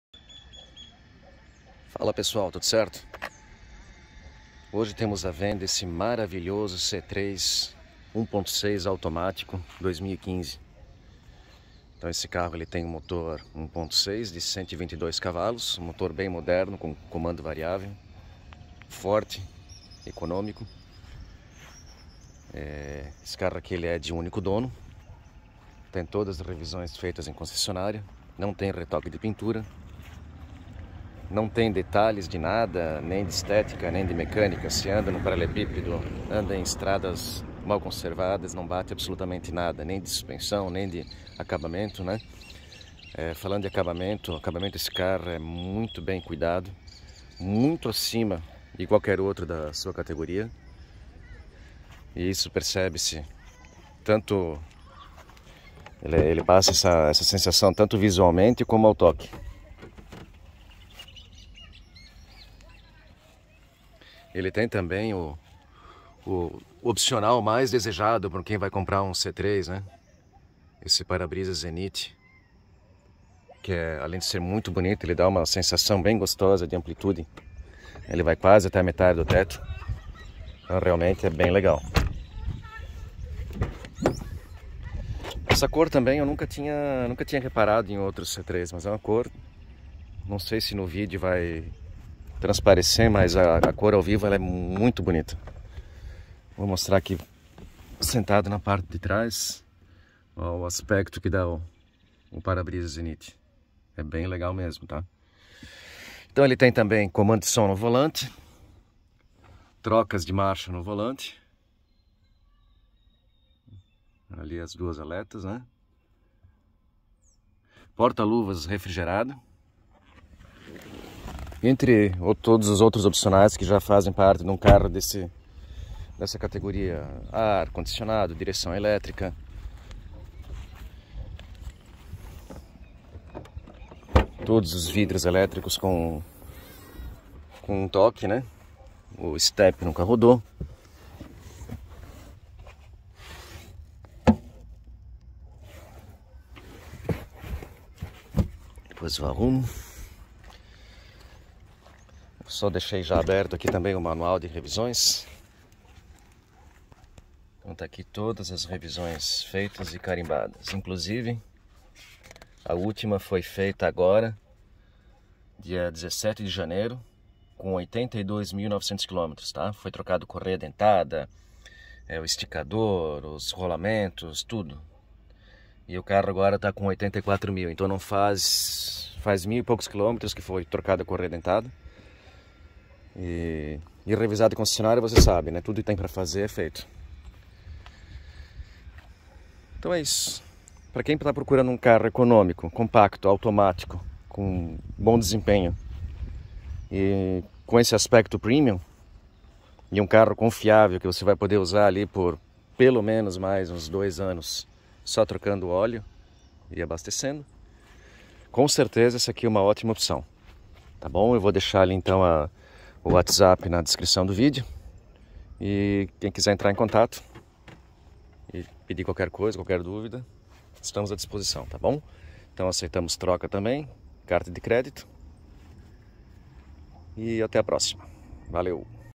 À Venda! C3 Tendance automático 2015
À Venda!

C3 Automático 2015

Único dono
Todas as revisões feitas na concessionária
Parabrisa Zenith
Controles de som no volante
Trocas de marcha no volante
Porta luvas refrigerado
Carro sem detalhes de uso, igual a zero km
IPVA 2022 pago!
KM 84.000
Aceito troca
Carta de crédito
Financia 100%